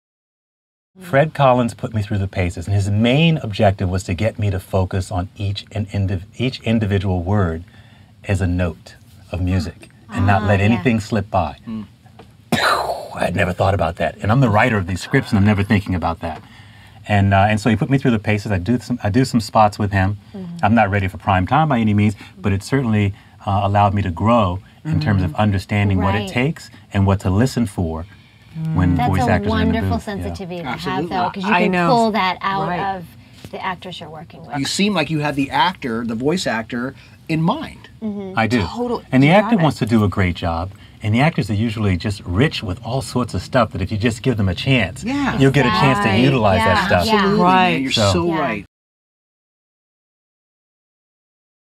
Rudy Gaskins - Treat Every Word Like A Note Of Music
VO Buzz Weekly Quick Tips. Voice over industry pros share tips and advice to help you grow your voice over business. This VO Buzz Weekly Quick Tip video features NY voice over writer, producer, director, Rudy Gaskins.